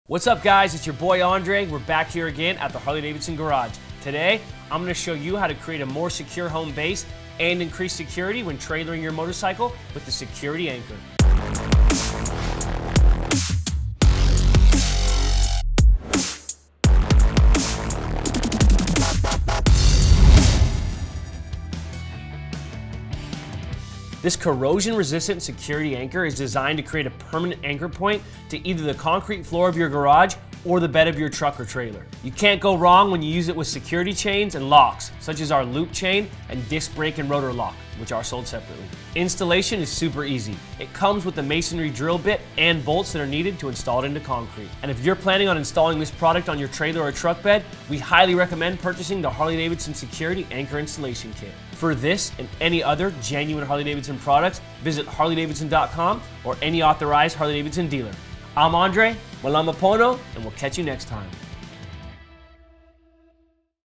Harley-Davidson Security Anchor Overview
Lock down your Harley-Davidson motorcycle. This rugged Harley-Davidson Security Anchor is designed to create a permanent anchor point for use with security chains and locks. Secure it to your truck or trailer bed or to a garage floor.

Includes masonry drill bit and bolts necessary to install into concrete. Recommended to be used with Harley-Davidson chains and locks, such as Loop Chain and Disc Brake & Rotor Lock.

Recommended to be used with Harley-Davidson Security Anchor Installation Kit for use on trailers or truck beds ➡️